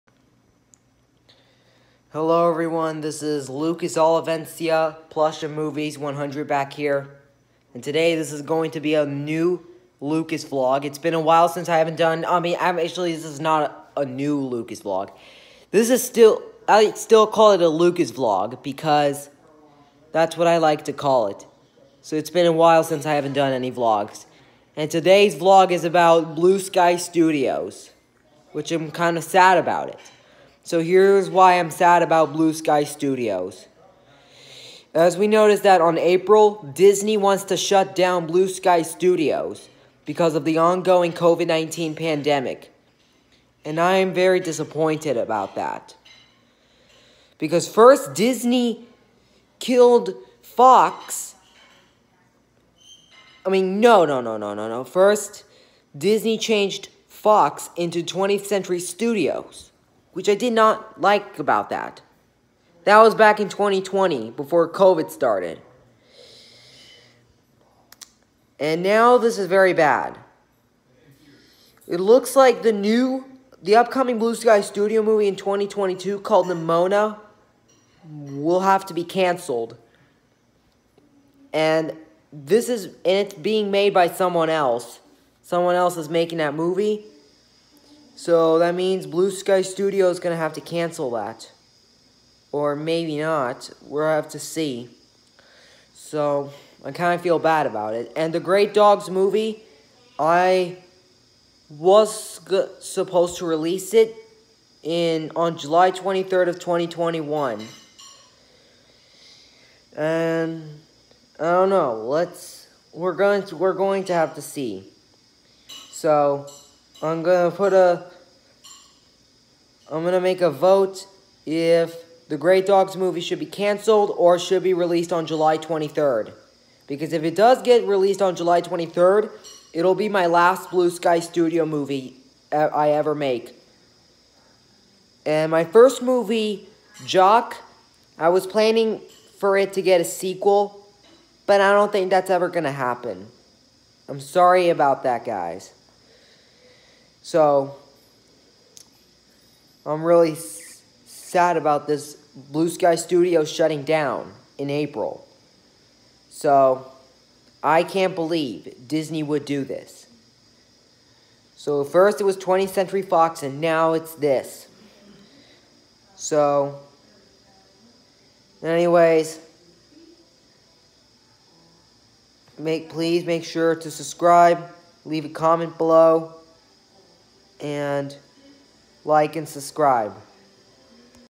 Lucas Vlog About Blue Sky Studios
So Guys, As You Know That On April Disney Decides To Shut Down Blue Sky Studios Which I Am Not Happy About And The Great Dogs Movie May Be My Last Blue Sky Studios Movie.

R.I.P Blue Sky Studios (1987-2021)